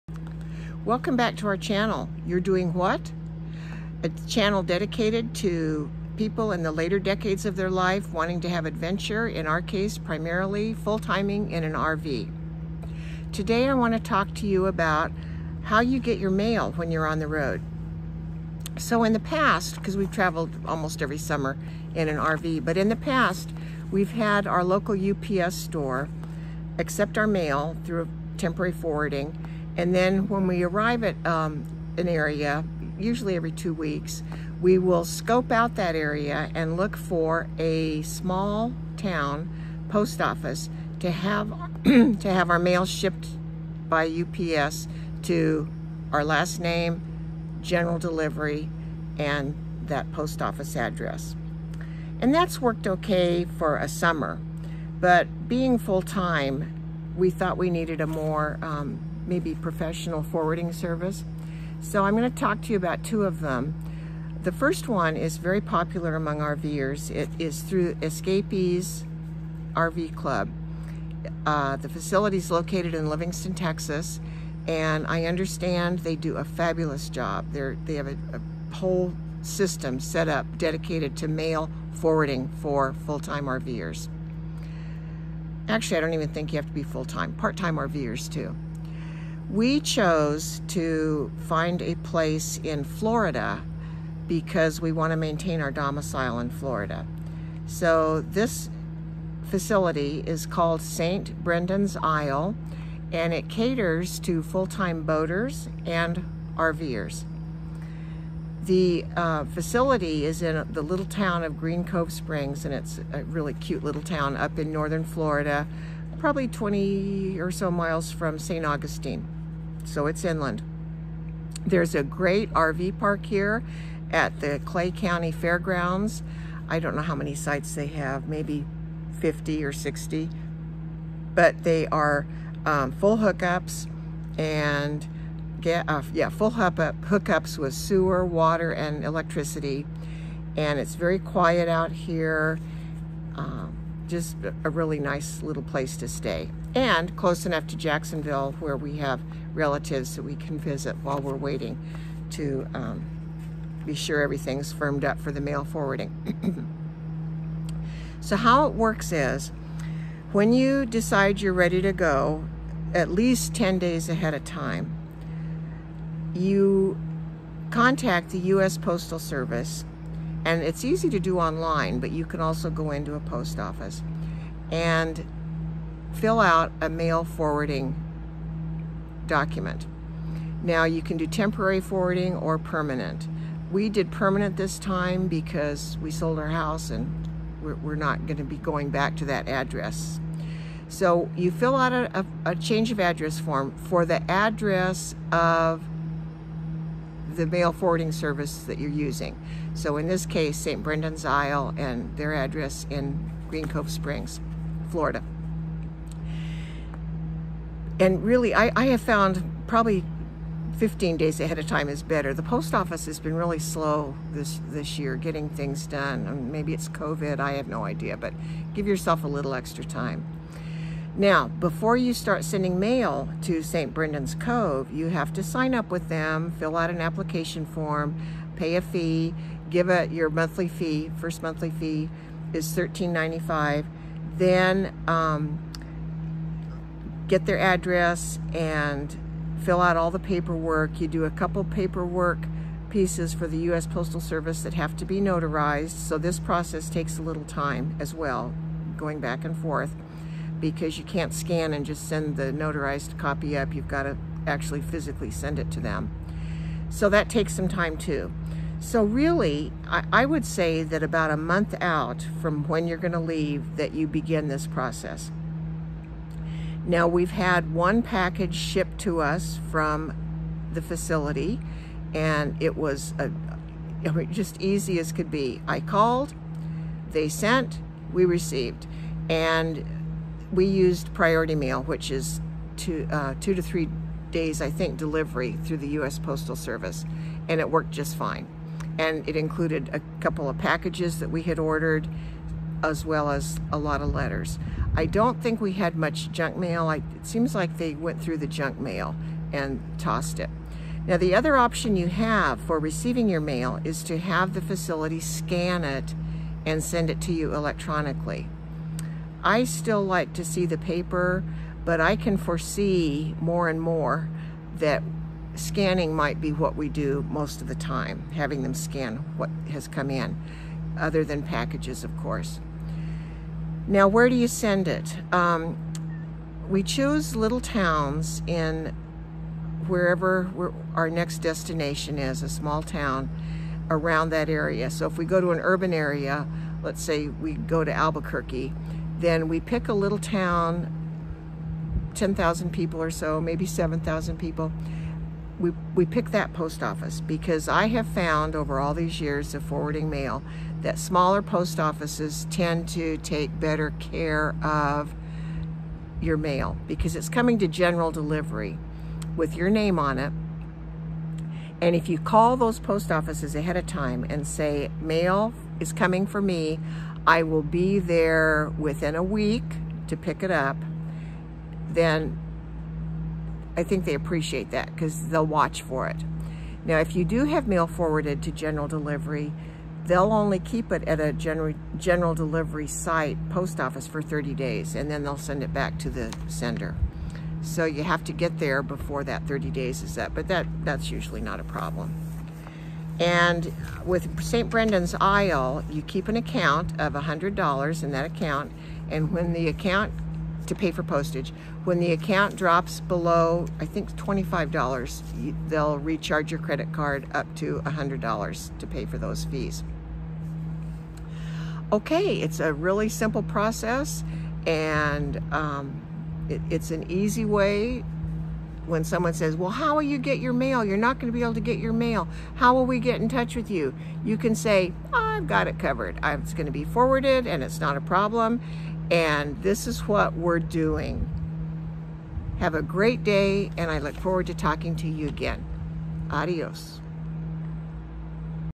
CAN WE GET OUR MAIL?????
This is a brief overview of the process and service we use to have our mail forwarded to us wherever we are.